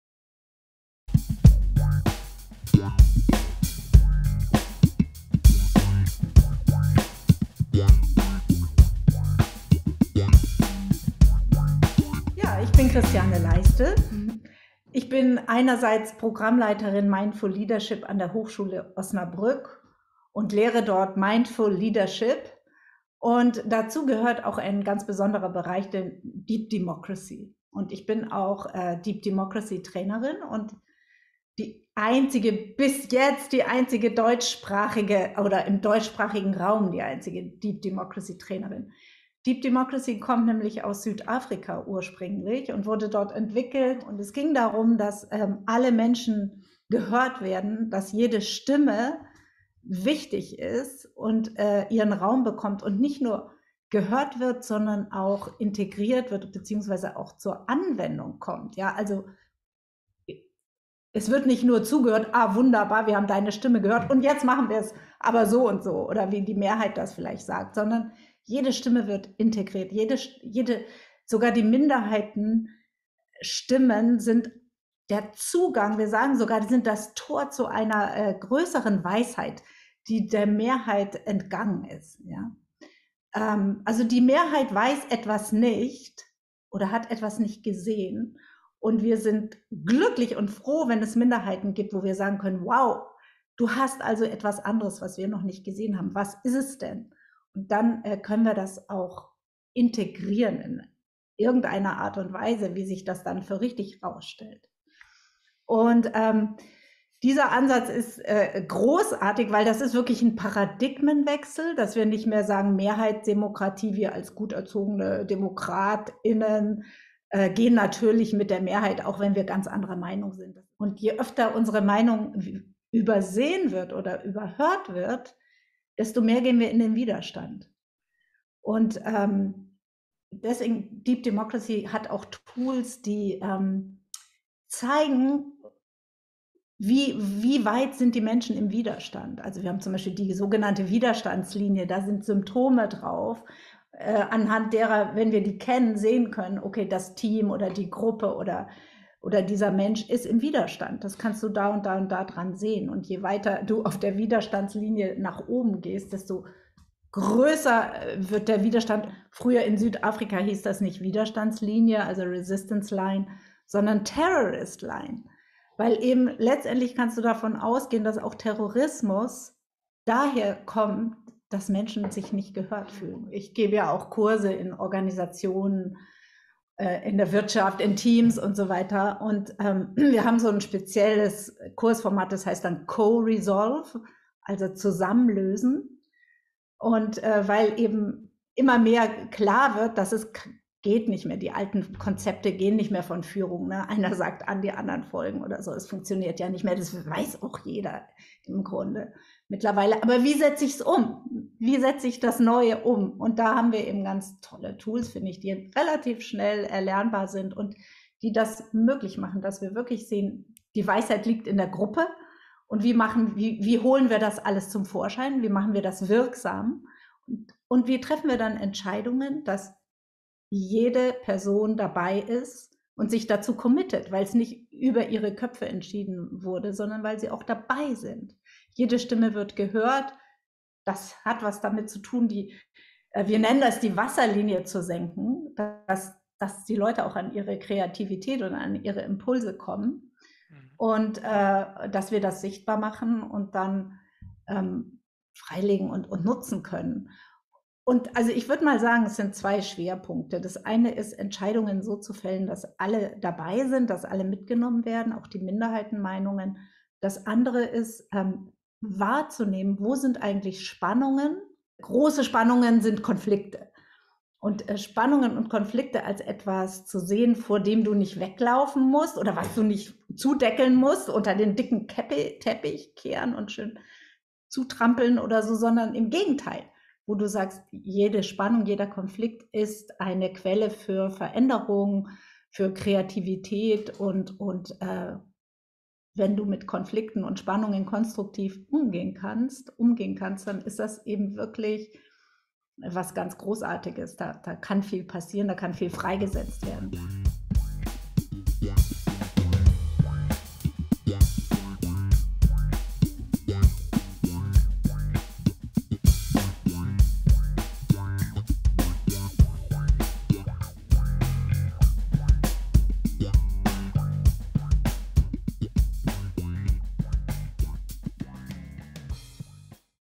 SyNA Tagung 2022: u.a. mit Christiane Leiste
Die Deep Democracy Trainerin Christiane Leiste erzählt über Aspekte von Deep Democracy, die sie auf der SyNA-Tagung 2022 in Bremen vertiefen wird.